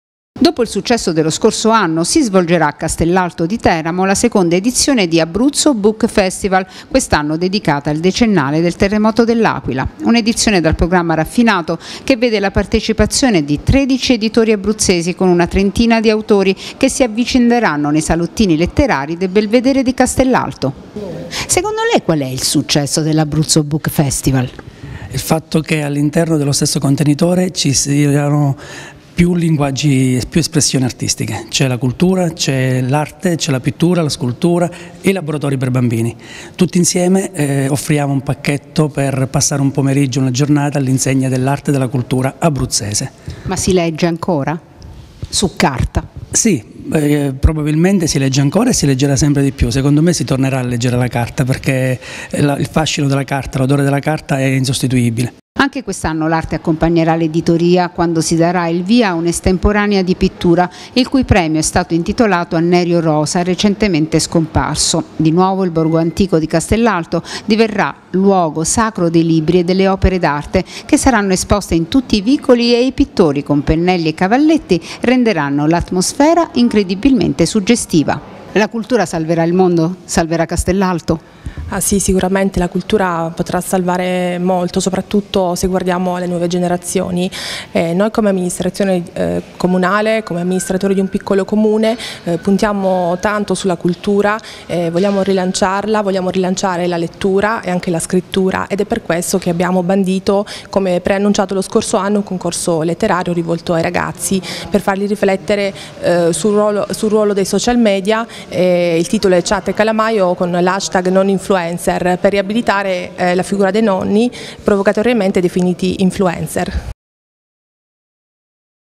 Abruzzo Book Festival: seconda edizione a Castellalto
Il 30 giugno prossimo, a Castellalto di Teramo, torna la seconda edizione dell'Abruzzo Book Festival con letture e laboratori per ragazzi. Libri e opere d'arte saranno esposti in tutti i vicoli del borgo teramano.